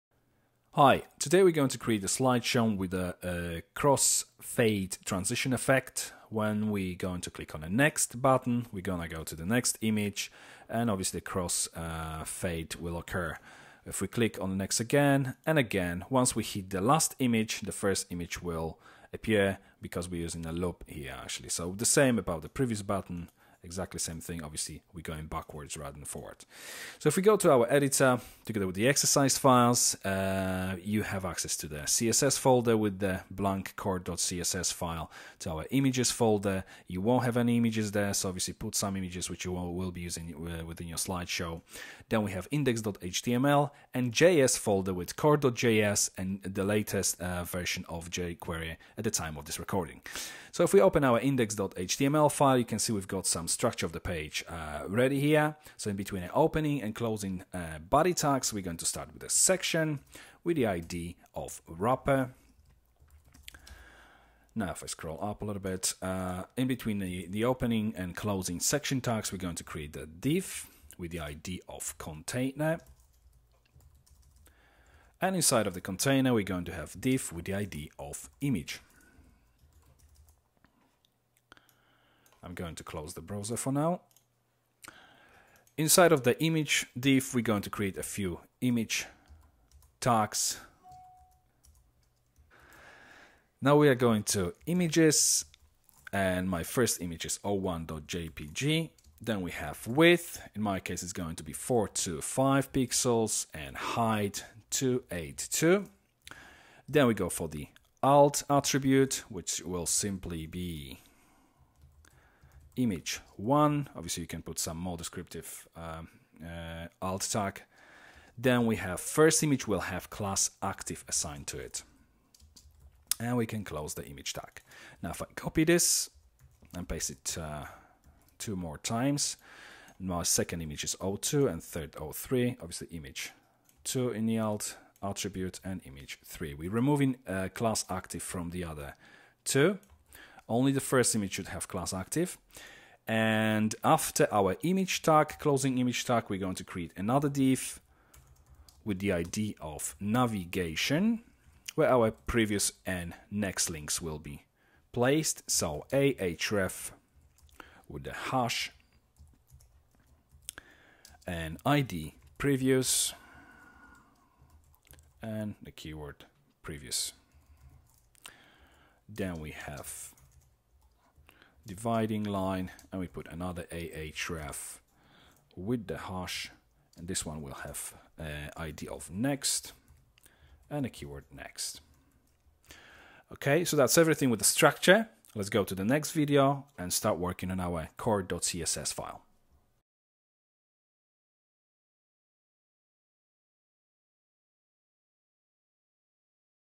Cross fade image slideshow with Previous and Next buttons
Learn how to create a cross fade image slideshow with Previous and Next buttons.